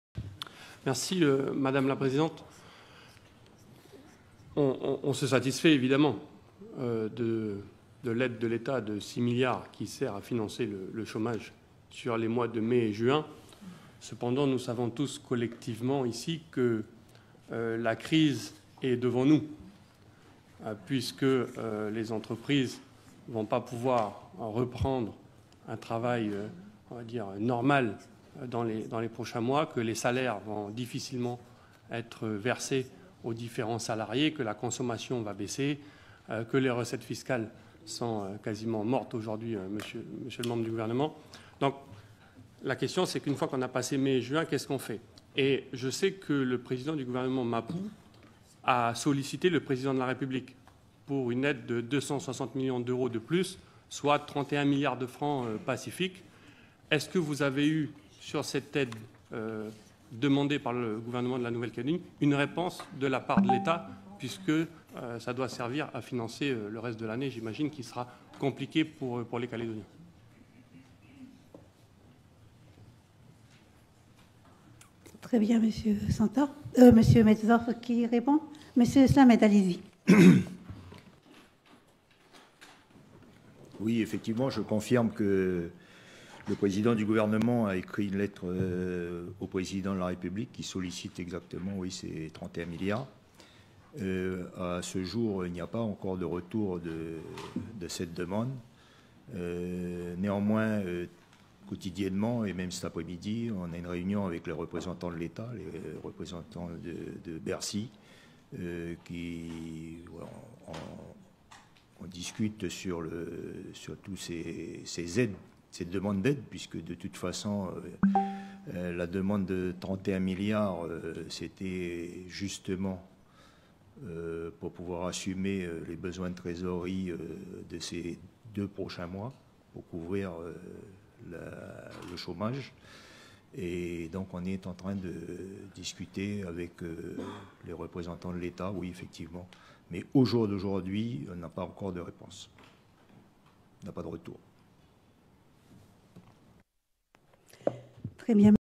2024.06.20-séance du congrès - inscription dans notre budget des 6 milliards de la France
Ce matin au congrès nous avons voté l’inscription dans notre budget des 6 milliards de l’aide de l’état pour financer le chômage partiel sur les mois de mai et juin.
Cependant la crise économique et donc sociale est devant nous et ces 6 milliards seront bien insuffisants. J’ai appris par Paris que le gouvernement avait sollicité une aide de 31 milliards (plus que les recettes de l’impôt sur le revenu sur un an) auprès du Président de la République.
En cas de réponse positive, même si elle ne serait pas effective d’un point de vue juridique, la mise sous tutelle serait « de facto » avec un tel versement, l’état conditionnant l’utilisation de cette manne financière à des dépenses qu’il aura identifiées.
Je vais œuvrer à mon niveau pour que la réponse soit positive. La survie de nos emplois en dépend.